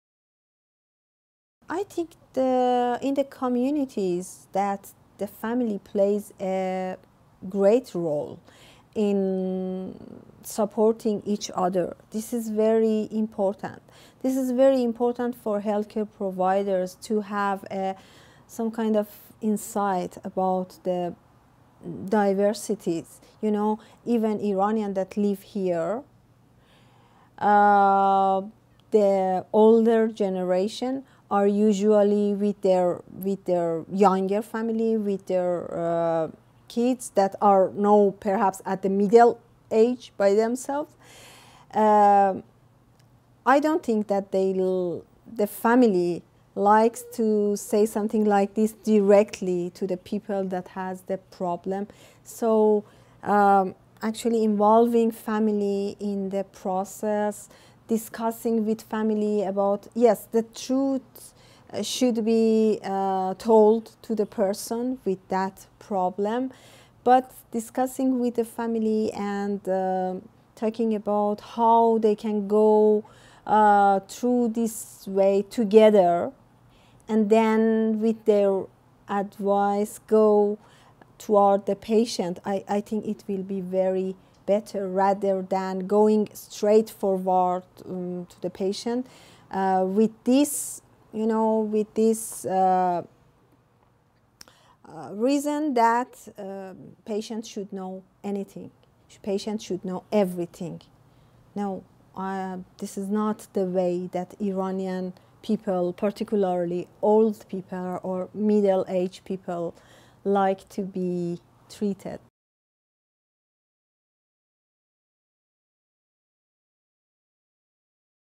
Soodi: Family first
In this video by Canadian Virtual Hospice, Soodie recommends involving the family in care decisions before approaching the patient.